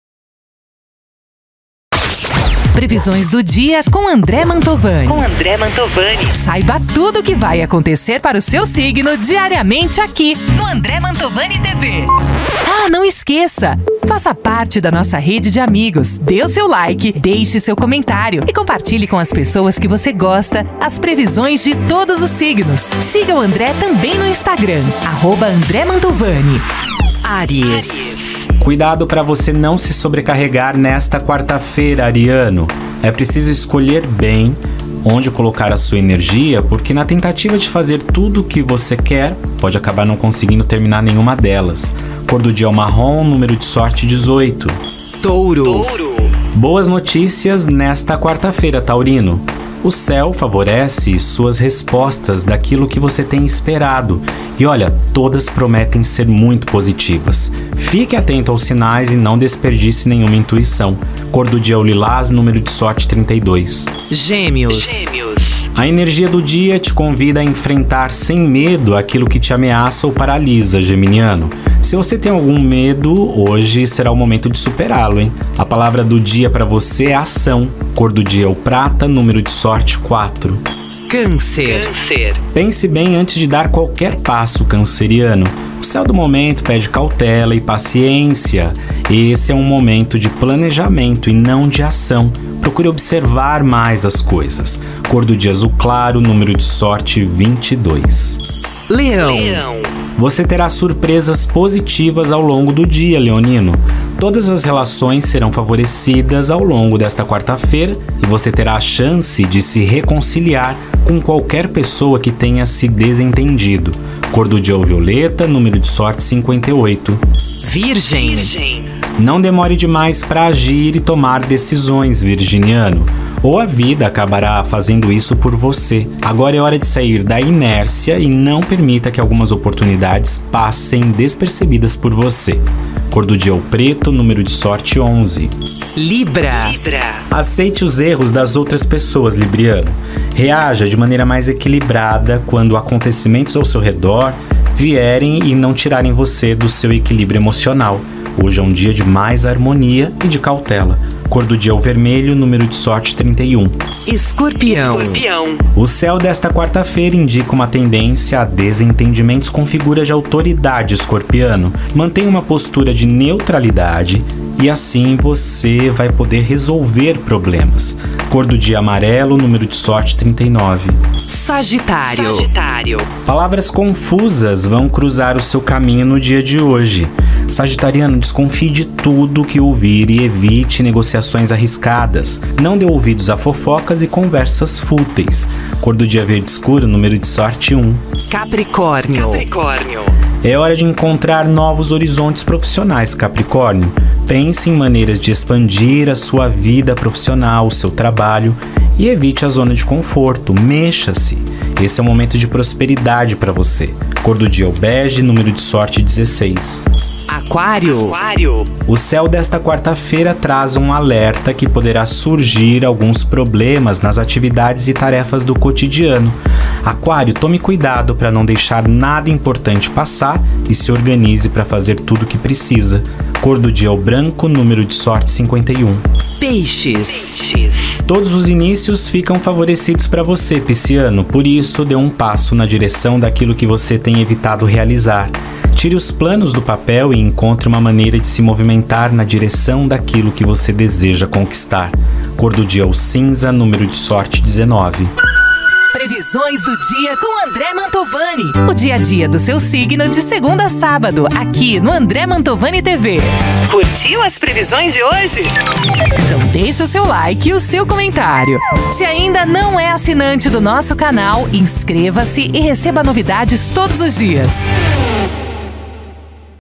HORÓSCOPO DO DIA - (QUARTA) - 17/08/2022 | André Mantovanni
HORÓSCOPO - Saiba as previsões de hoje para todos os signos!

💡Faça 2 perguntas para o André Mantovanni e receba as respostas em áudio!